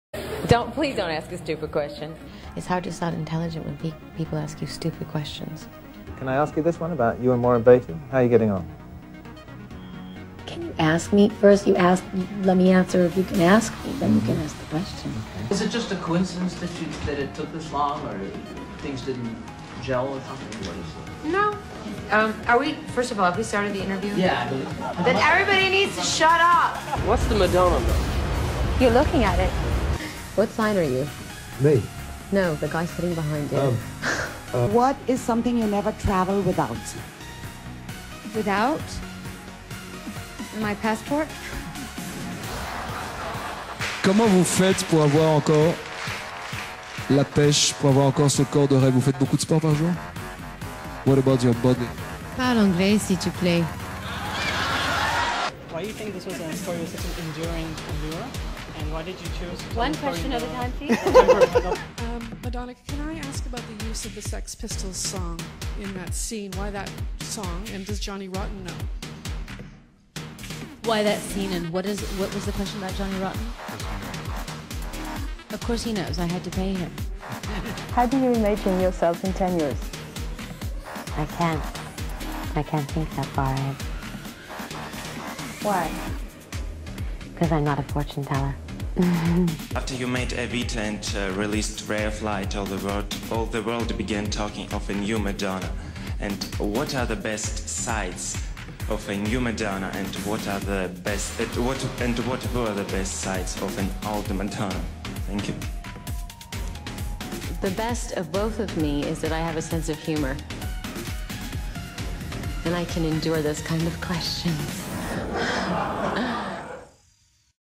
MADONNA: Respostas cretinas para perguntas idiotas
Sempre me divirto muito com esta compilação de várias respostas da Madonna para alguns jornalistas e apresentadores, então achei que seria interessante postar aqui também no canal.

Os créditos para a montagem e criação do vídeo são inteiramente da página POSER DE MADONNA.
Conheçam! Na minha opinião é a melhor página sobre Madonna no Facebook!